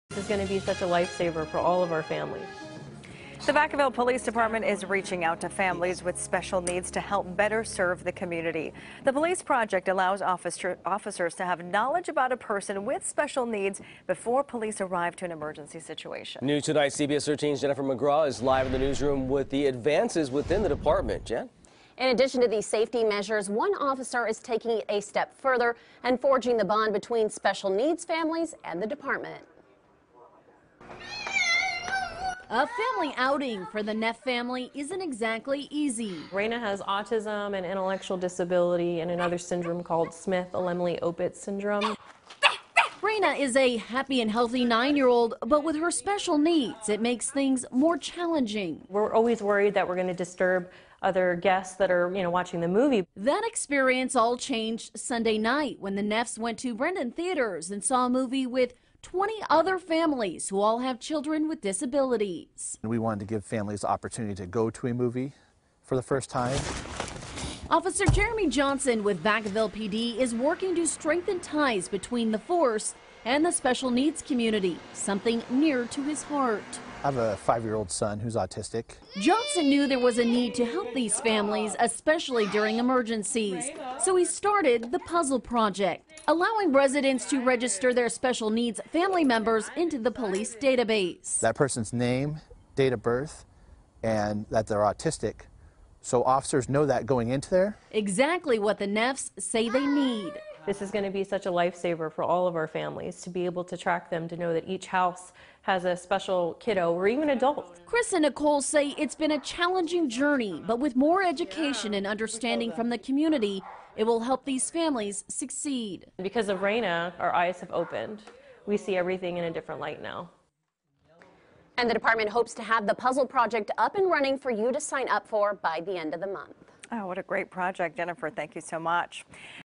Vacaville Police Reaching Out To Help Families With Special-Needs Children
Officer Jeremy Johnson knew there was a need to help these families, especially during emergencies, so he started the "Puzzle Project," allowing residents to register their special-needs family members into the police database.